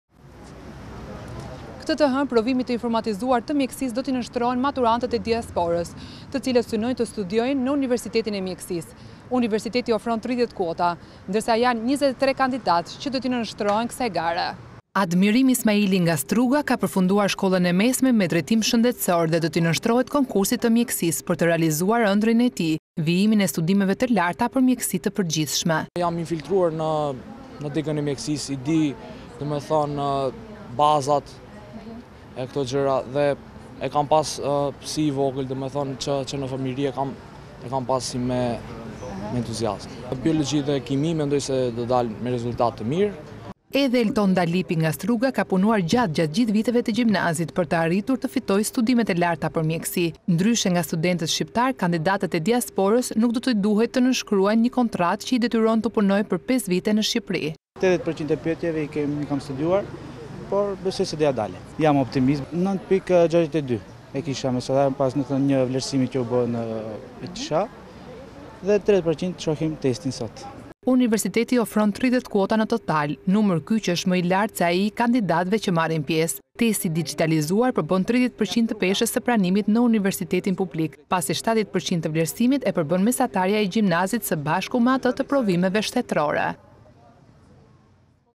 Konkursi për mjekësinë, në testim 23 kandidatë nga diaspora, 30 vende të lira | RTSH
Gazetare: K.Toroveci